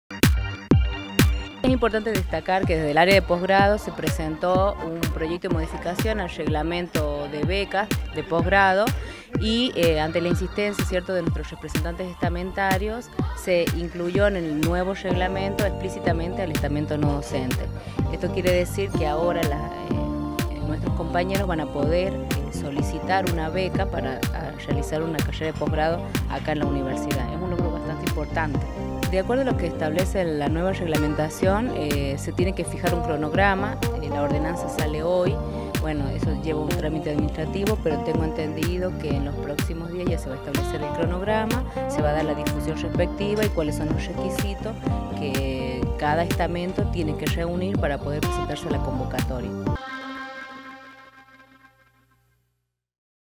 U Noticias Nodocentes podrán acceder por primera vez a becas de posgrados
El Consejo Superior de la UNLaR modificó la ordenanza que regula las becas de posgrado y por primera vez se incorpora el estamento no docente. Ahora lxs compañerxs podrán ser aspirantes beneficiarixs de las becas de posgrado, al igual que docentes y graduadxs que tengan título de grado.